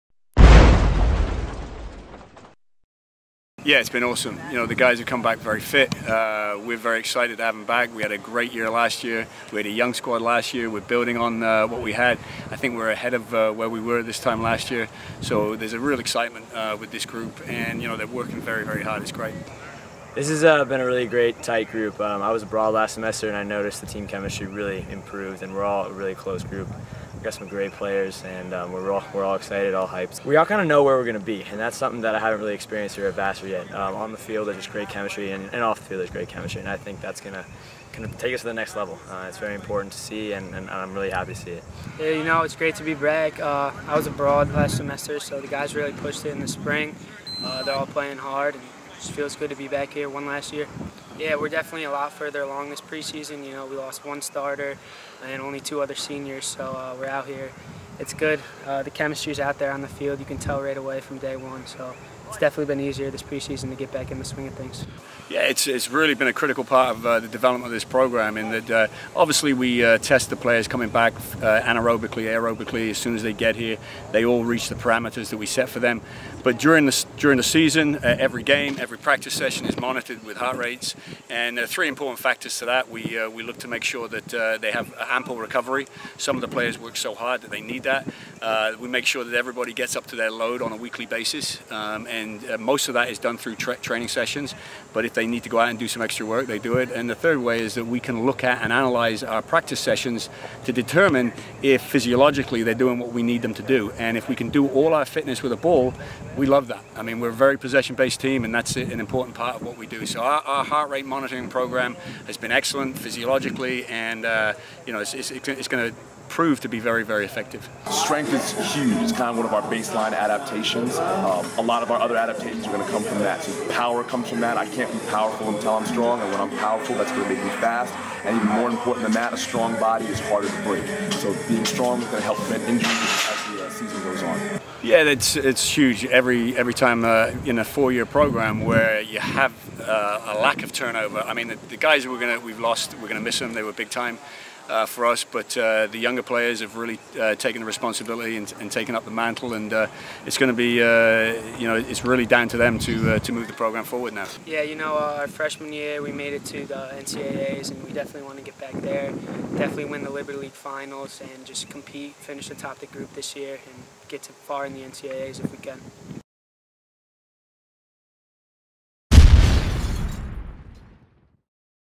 2015 Vassar College Men's Soccer Preview
Check out what head coach Andy Jennings and senior captains Jordan Palmer and Nick Hess had to say about the team's upcoming 2015 season